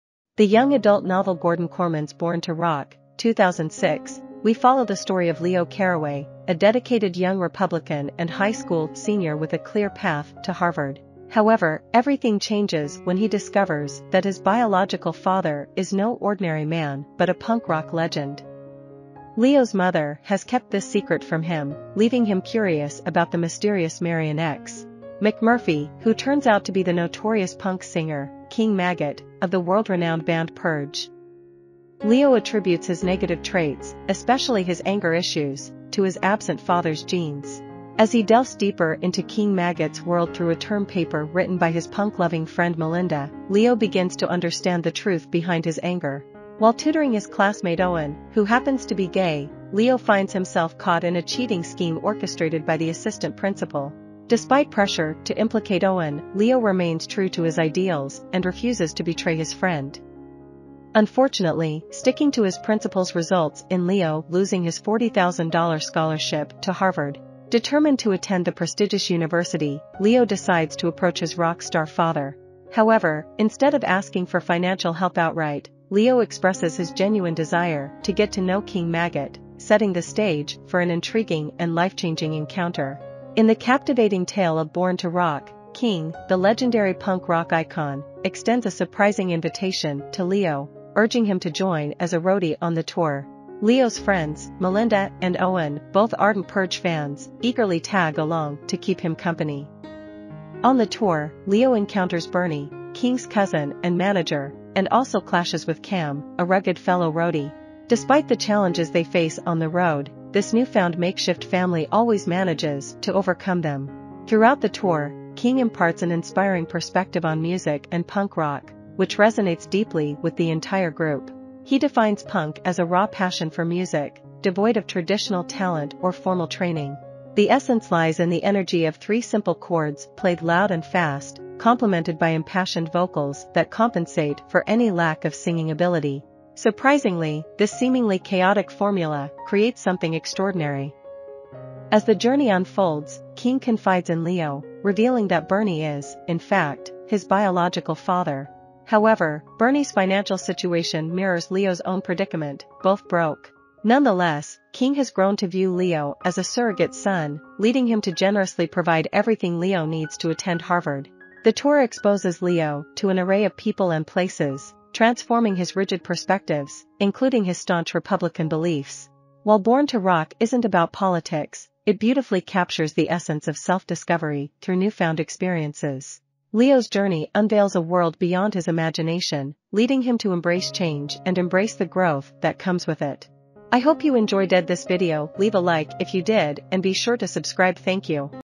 Plot summary, “Born to Rock” by Gordon Korman in 3 Minutes - Book Review
"Born to Rock" is a young adult novel written by Gordon Korman. It was first published in 2006. Gordon Korman is a prolific author known for his engaging and humorous stories for young readers.
The story follows the life of Leo Caraway, a seventeen-year-old high school senior who is the quintessential "good kid." He has always been responsible and dutiful, striving to achieve academic excellence and make his parents proud. Leo's parents are both professors, and they expect him to follow in their footsteps by attending Harvard University.
However, Leo's life takes an unexpected turn when he discovers that he was adopted and that his birth parents were none other than King Maggot, the lead singer of a legendary punk rock band, and Queen Duf, the band's bass player. This revelation shatters the image of the perfect, studious son he thought he was.
Intrigued by his newfound heritage, Leo decides to delve into the world of punk rock and sets out on a quest to find his birth parents. He leaves his safe and conventional life behind to explore the rebellious and chaotic world of his punk rock roots.
Throughout the novel, Leo's journey is filled with humorous and sometimes challenging situations as he embraces his punk rock identity and learns more about his birth parents and their unconventional lifestyle. The book explores themes of identity, family, self-discovery, and the idea that sometimes, the path we choose might not align with the expectations others have for us.
Gordon Korman's writing style is known for its wit and relatable characters, making "Born to Rock" an entertaining and engaging read for young adult audiences. It celebrates the spirit of individuality and the importance of staying true to oneself, even when faced with societal pressures and expectations.
If you're a fan of coming-of-age stories with humor, music, and themes of self-discovery, "Born to Rock" might be an enjoyable addition to your reading list.